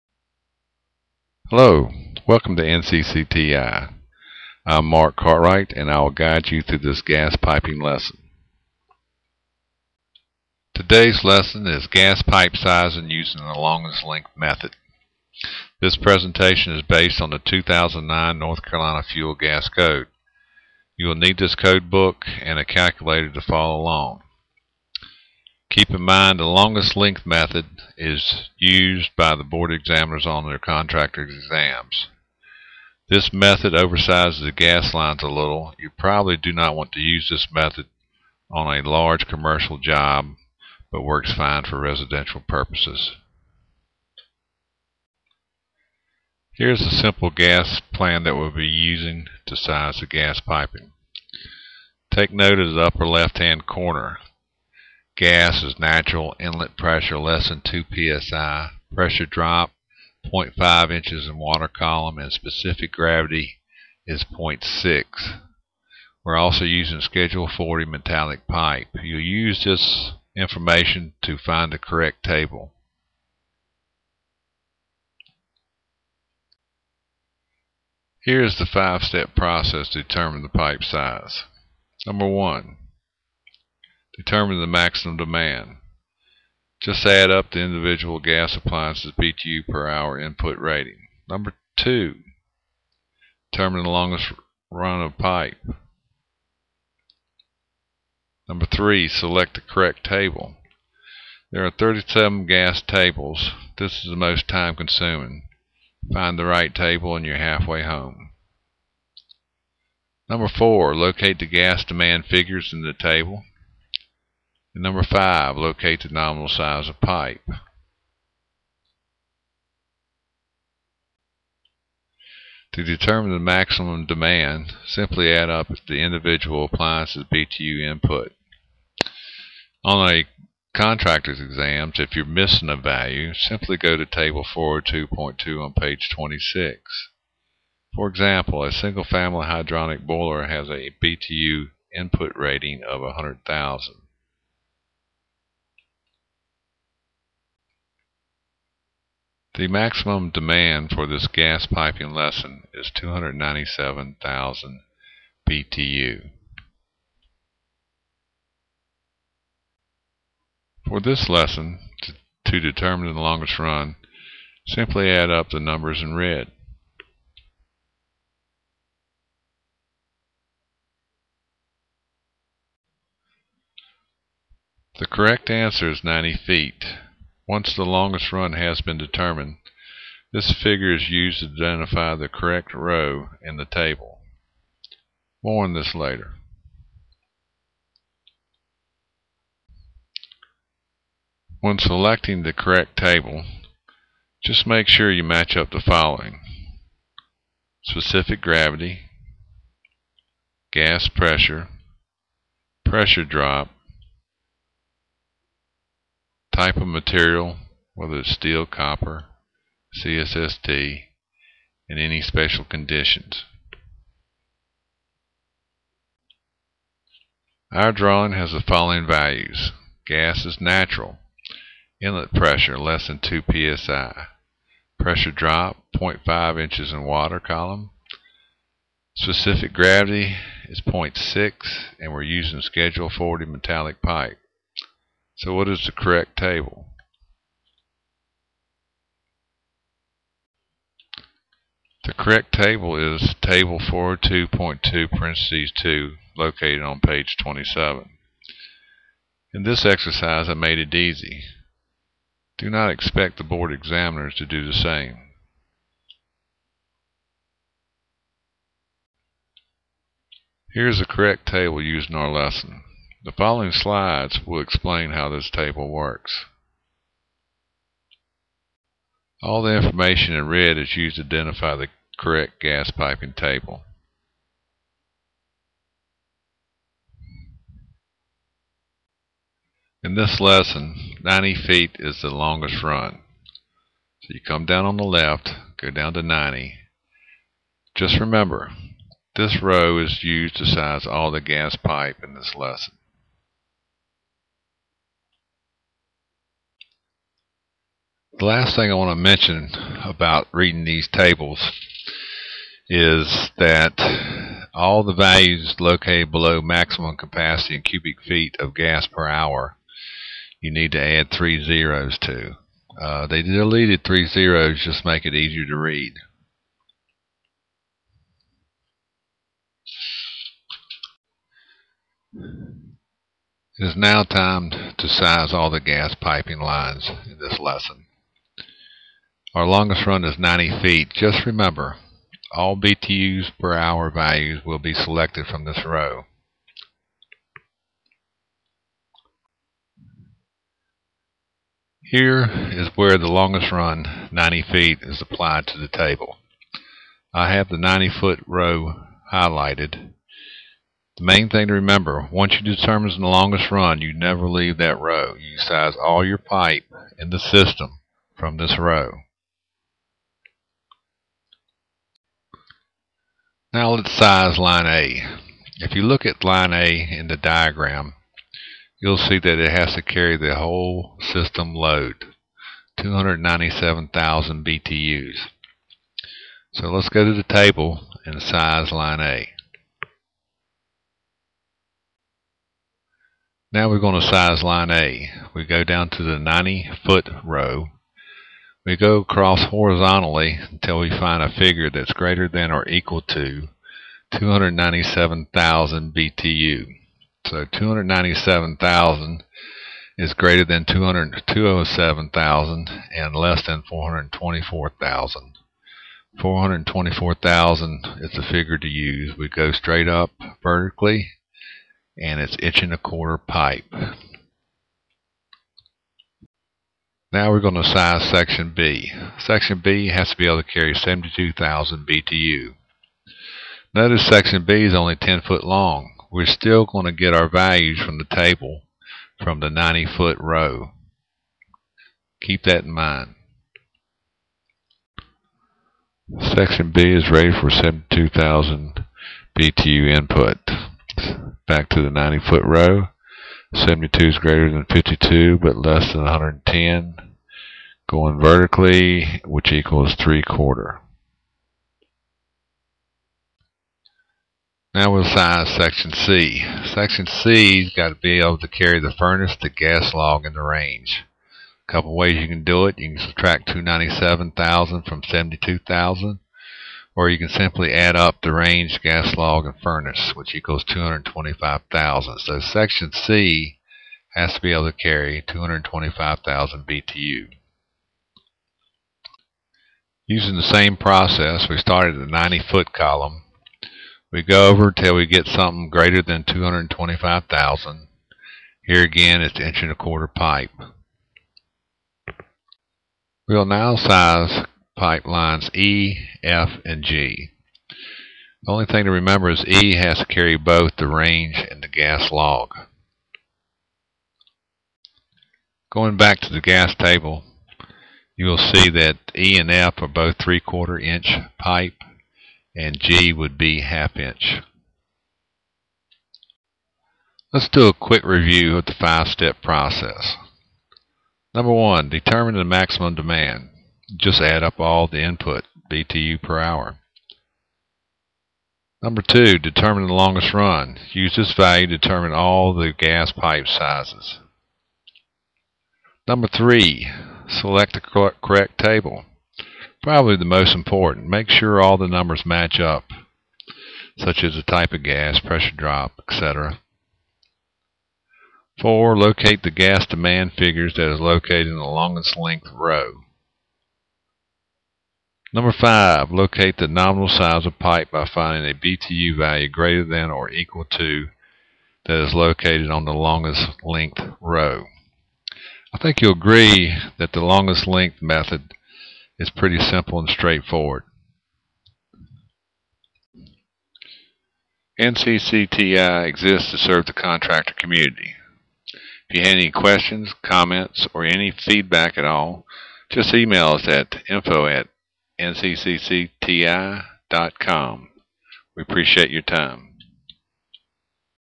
Longest lenght method.flv
The Longest Length Method is used to size fuel gas piping. This presentation is based on the  2009 North Carolina Fuel Gas Code book. North Carolina Contractor Testing Institute created this lesson to prepare students to pass the North Carolina Board of Examiners' Contractor exams. Being able to size gas piping is one of the core competencies required for the Heating, Plumbing, and Fuel Piping Contractor Exams.